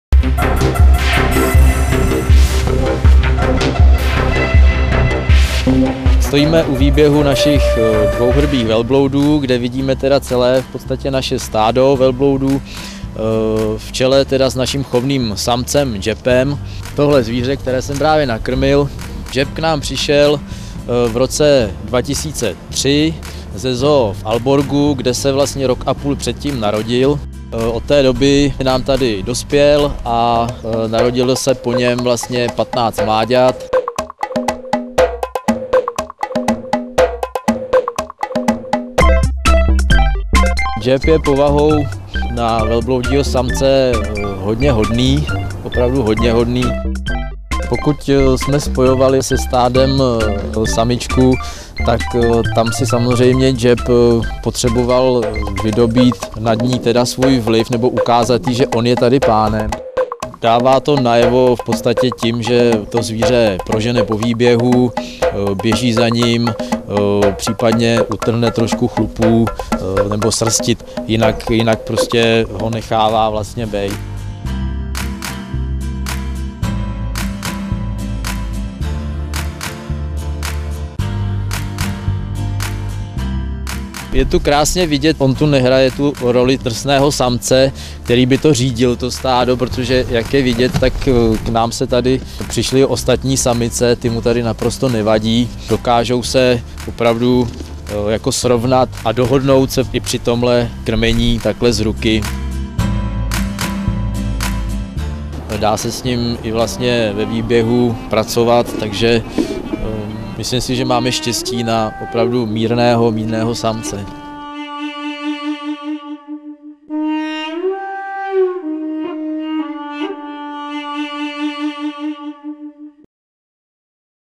Seznamte se s Jepem!
Samec velblouda dvouhrbého si jistě zaslouží být osobností kampaně Seznamte se! Má nesmírně přátelskou povahu, a to jak k ostatním velbloudům, tak k lidem. V Praze žije od roku 2003 a od té doby má na svém kontě už pěknou řádku mláďat!